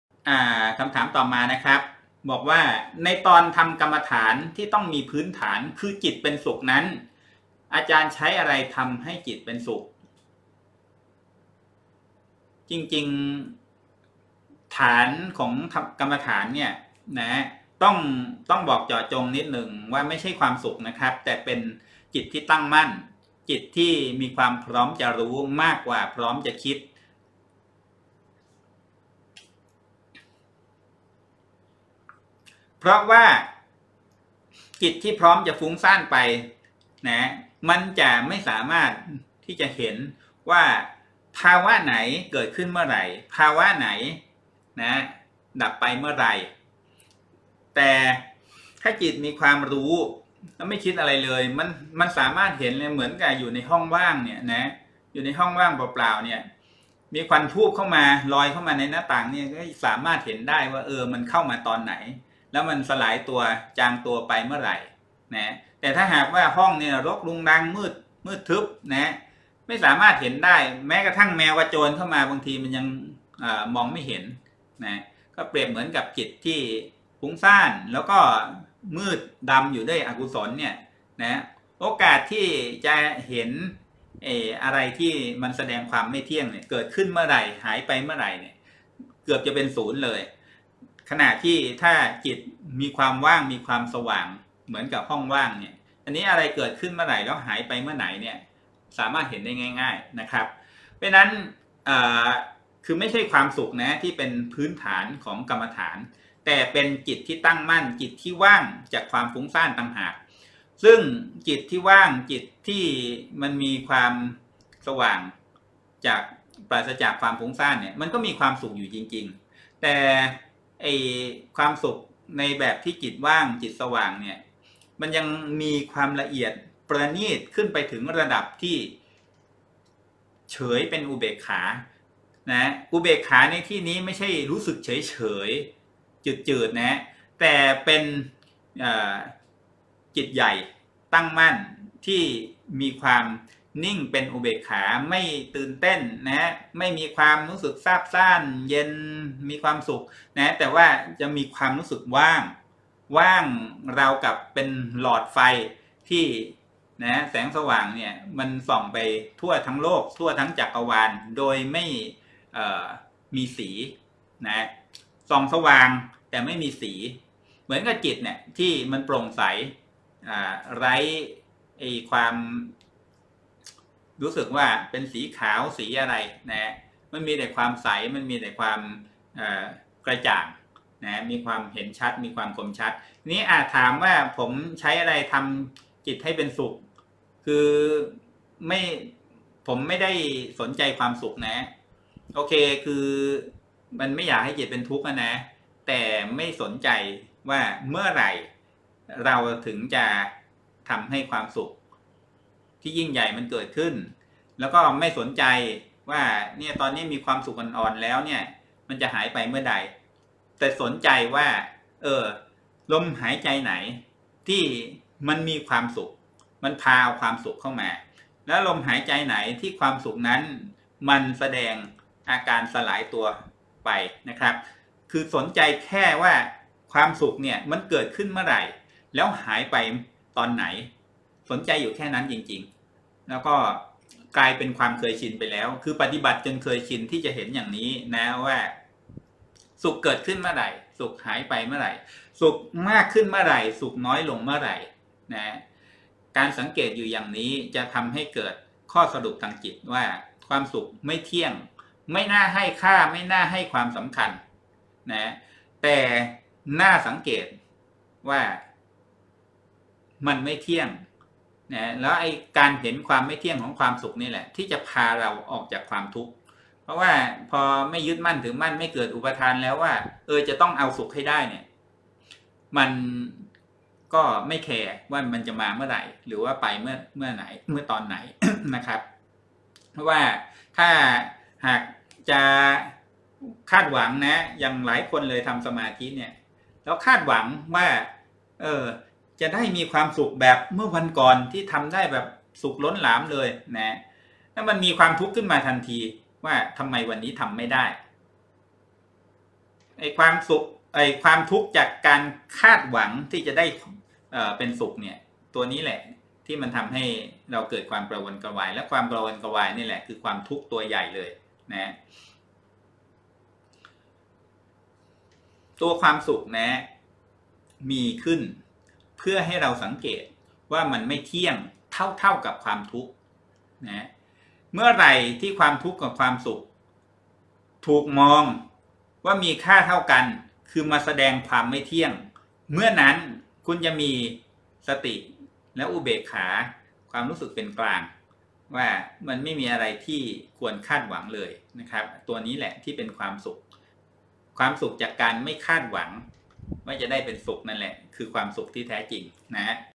ในตอนทำกรรมฐาน ที่ต้องมีพื้นฐาน คือจิตเป็นสุขนั้น อาจารย์ใช้อะไร ทำให้จิตเป็นสุขครับ?
ปฏิบัติธรรมที่บ้าน ตอน สวดมนต์ข้ามปี ชีวิตจะดีจริงไหม?
▶▶ คำถามช่วง – ถามตอบ ◀◀
29 ธันวาคม 2561